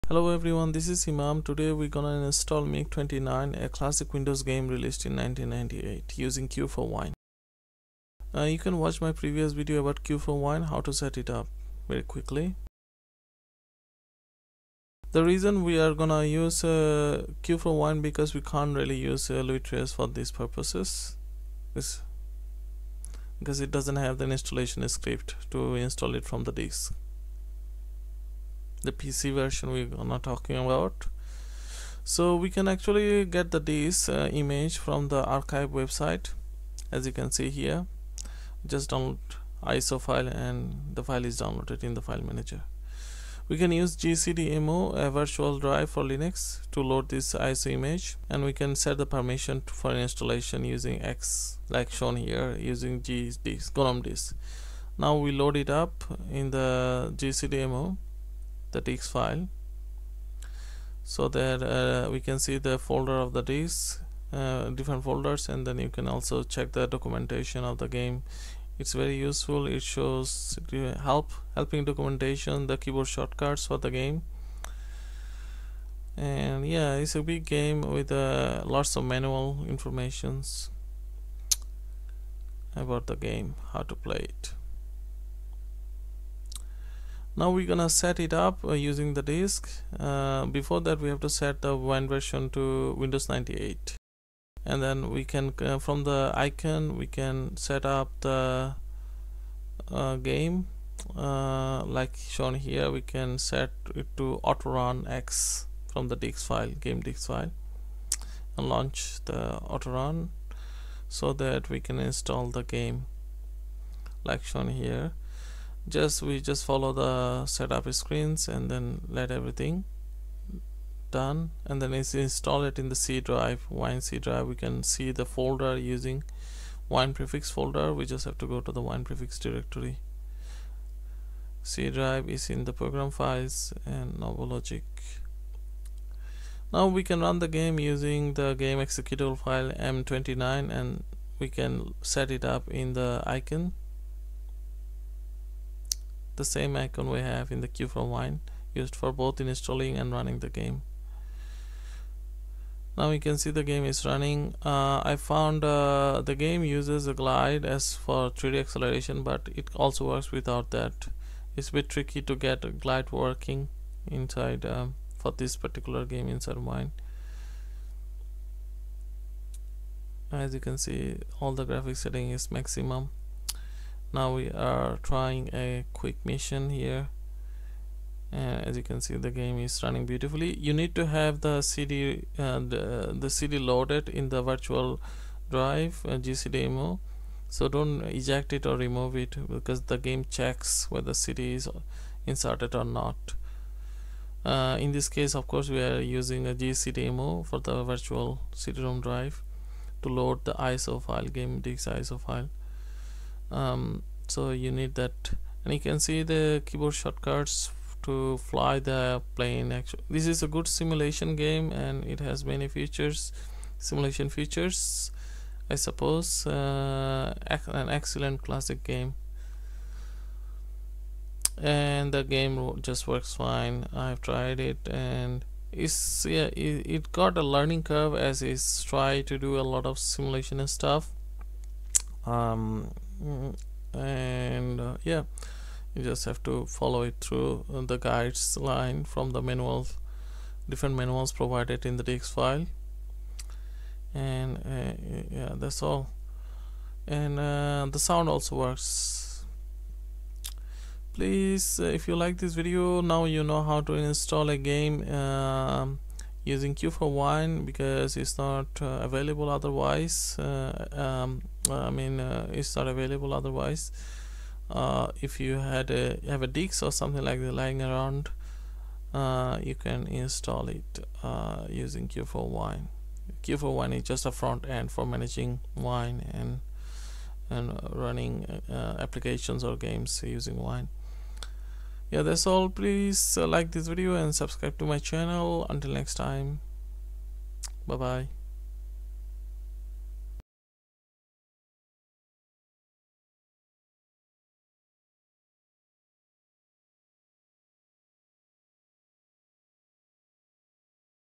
Install & Play 90s Windows Games on Linux – Step-by-Step Tutorial
This is the easiest way to play old Windows games on Linux.

My PC:

AMD Ryzen 5 5600X 6-Core Processor @ 3.7GHz
16 GB RAM